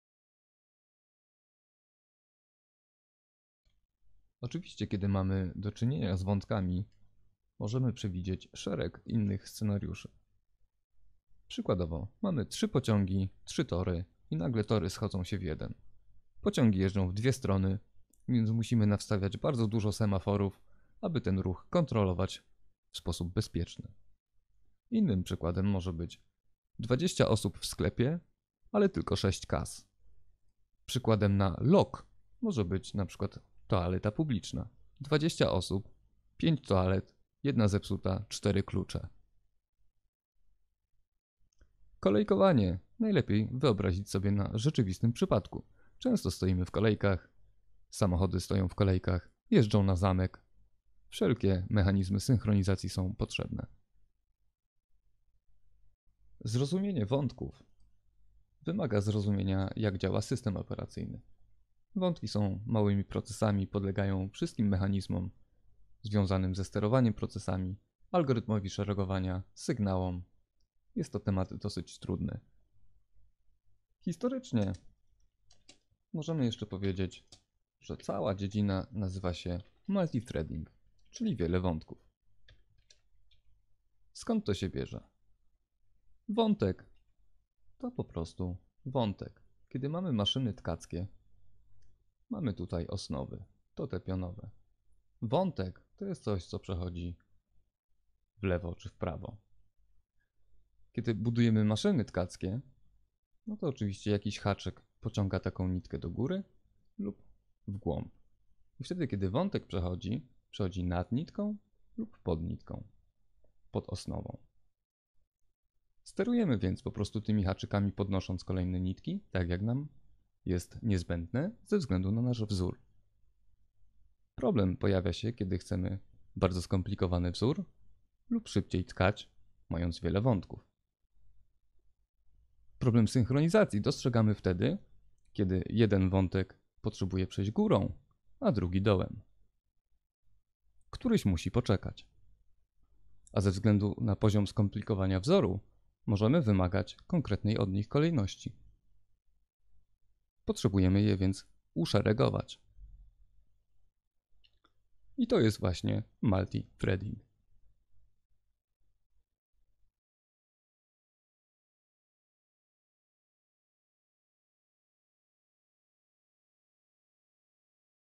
Python 3: programowanie współbieżne: podsumowanie.
Po przebrnięciu przez serię ciekawych wyzwań z domeny programowania współbieżnego, i po zbudowaniu własnej maszyny wirtualnej, wracamy do
prostego pytania: czym i po co jest multi-threading.